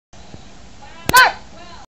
Movie 1337
He I are embarassing my sister Meridith I animate and she says "FART!"